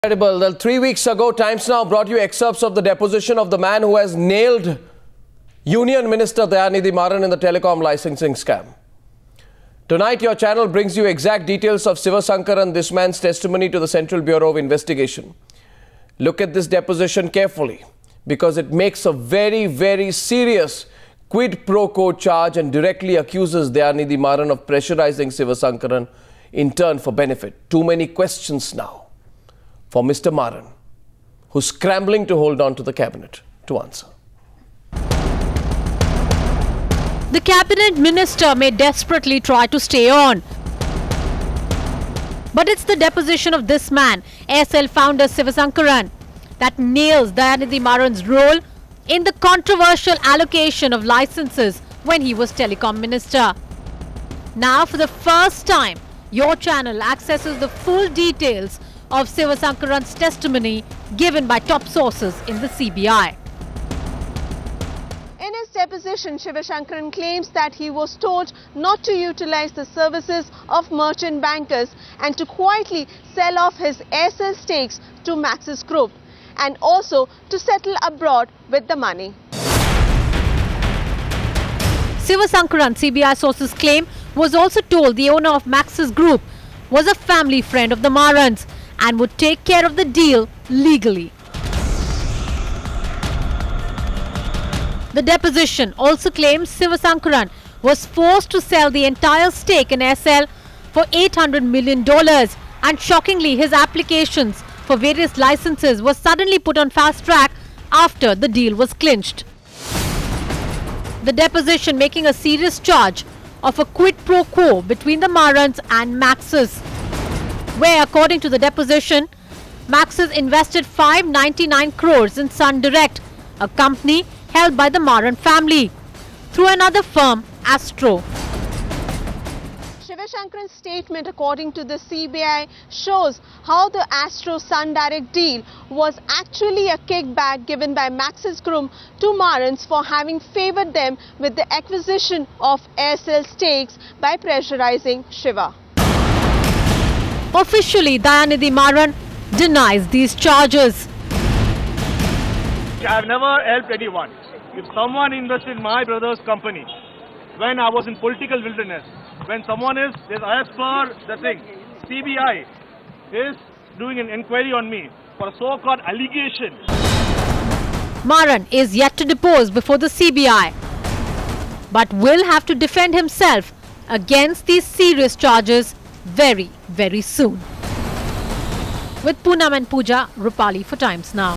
Sivasankaran alleges pressure by Dayanidhi Maran
TIMES NOW has accessed full details of former Aircel chief C Sivasankaran's deposition to CBI in the 2G spectrum scam. As per the testimony details, Sivasankaran alleges that he was forced to sell Aircel to Maxis, a company owned by alleged family friend of Marans at a time when now textiles minister Dayanidhi Maran was the telecom minister. Download the Times Now India's Election HQ app and get all the election info at one go. Social Media Links